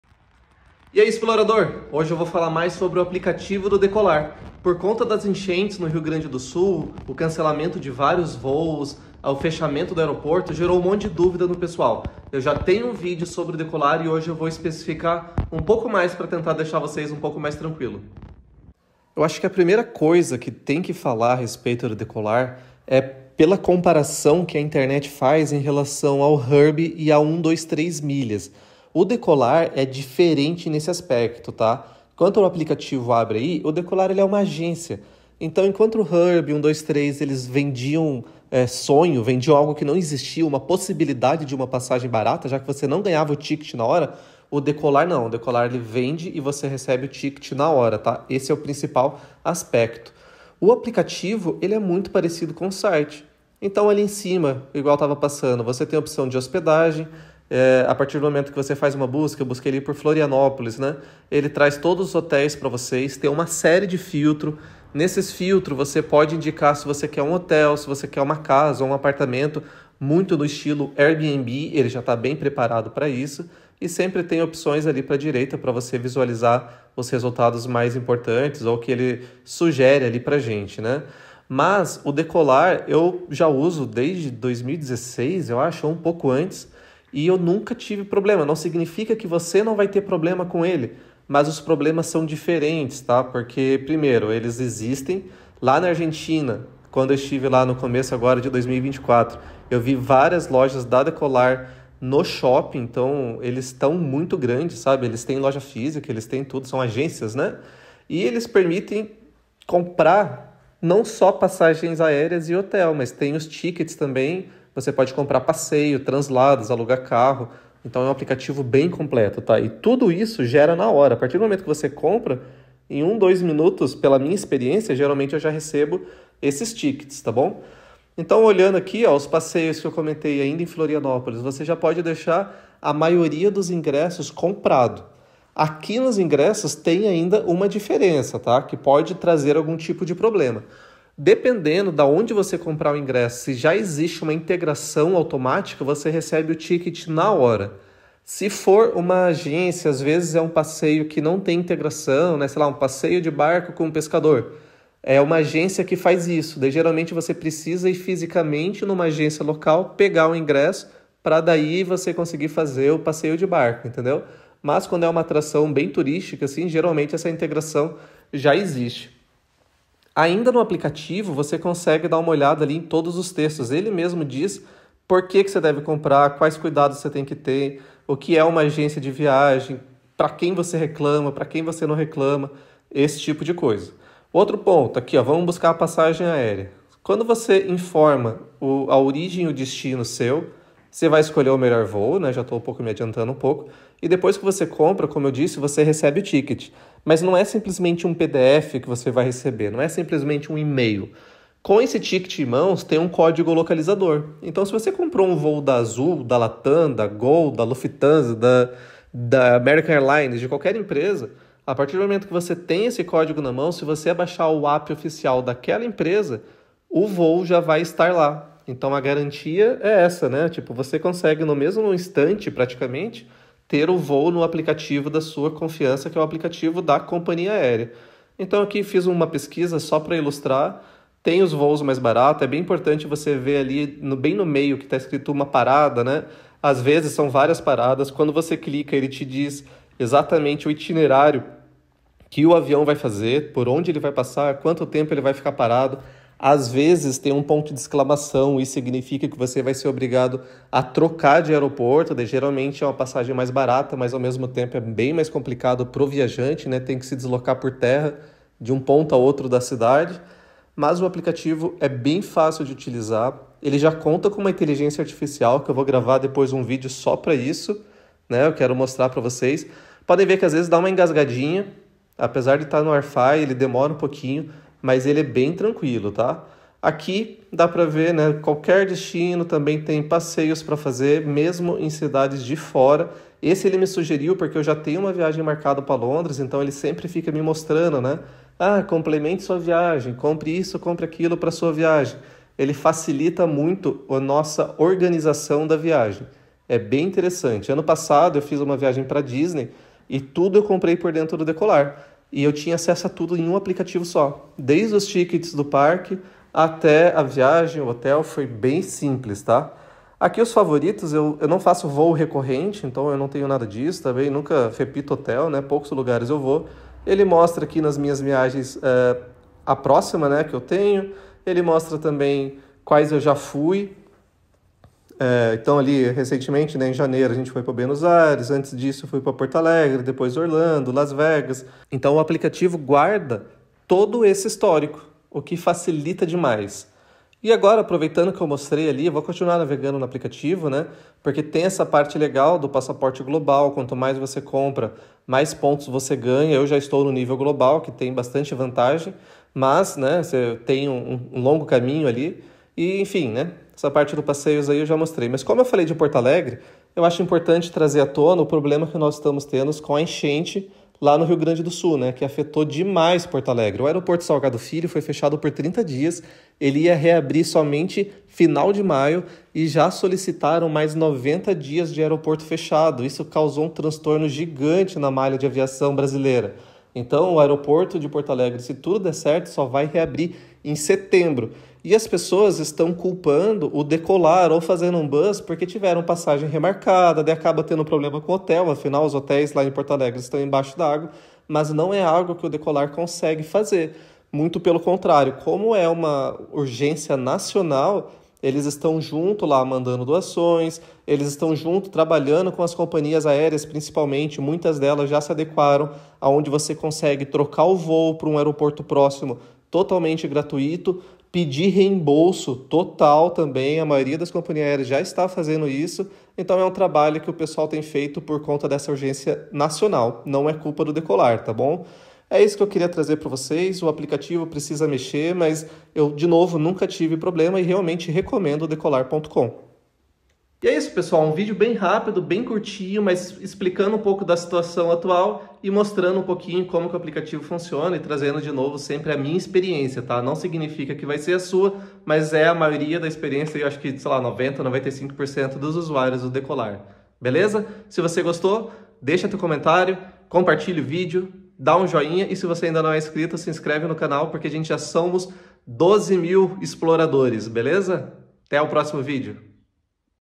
VALE A PENA COMPRAR NO DECOLAR PELO APP?
Fala Exploradores! As fortes chuvas que atingiram o Rio Grande do Sul causaram uma tragédia, com Porto Alegre literalmente debaixo d'água! O Aeroporto Salgado Filho está fechado e MUITA gente está com viagens aéreas canceladas. 😥

E o pior: estão culpando o DECOLAR pelos cancelamentos! Mas será que a culpa é realmente do aplicativo? 🤔

Nesse vídeo, eu explico toda a situação: mostro o caos que a enchente causou, como isso impactou o Aeroporto Salgado Filho e esclareço de uma vez por todas se a culpa é do app, das cias aéreas ou dos hotéis.

Se você está com viagem marcada para Porto Alegre nos próximos meses, precisa assistir esse vídeo! ⚠️

Ah, e não esquece de deixar seu comentário com a sua opinião sobre essa situação!